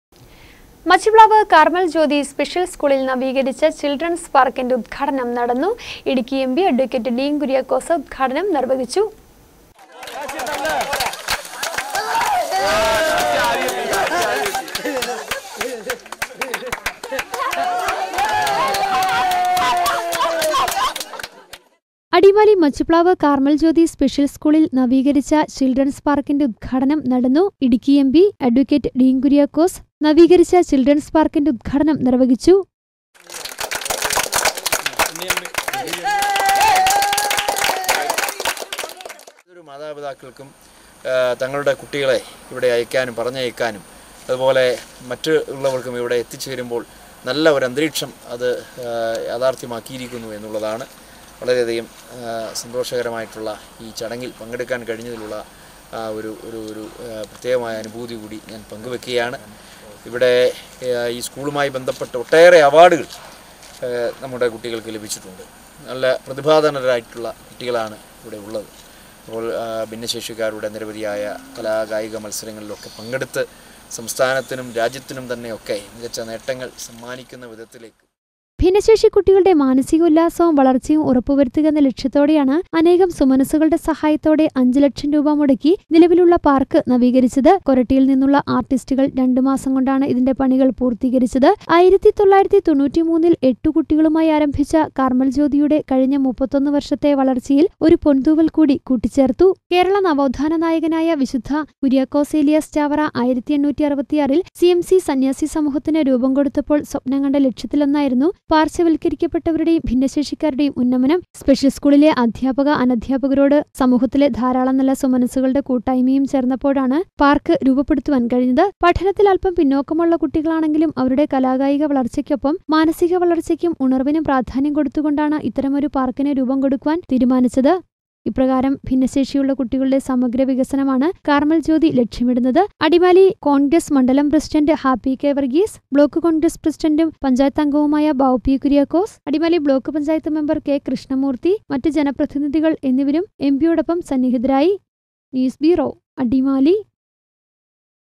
മച്ചിപ്ലാവ് കാര്‍മ്മല്‍ ജ്യോതി സ്‌പെഷ്യല്‍ സ്‌കൂളില്‍ ചില്‍ഡ്രന്‍സ് പാര്‍ക്കിന്റെ ഉദ്ഘാടനം നടന്നു
മച്ചിപ്ലാവ് കാര്‍മ്മല്‍ ജ്യോതി സ്‌പെഷ്യല്‍ സ്‌കൂളില്‍ നവീകരിച്ച ചില്‍ഡ്രന്‍സ് പാര്‍ക്കിന്റെ ഉദ്ഘാടനം ഇടുക്കി എം.പി  ഡീന്‍ കുര്യാക്കോസ് നിര്‍വഹിച്ചു.ഭിന്നശേഷി കുട്ടികളുടെ മാനസിക ഉല്ലാസവും വളര്‍ച്ചയും ഉറപ്പുവരുത്തുക എന്ന ലക്ഷ്യത്തോടെയാണ് അനേകം സുമനസ്സുകളുടെ സഹായത്തോടെ 5 ലക്ഷം രൂപ മുടക്കി നിലവിലുള്ള പാര്‍ക്ക് നവീകരിച്ചത്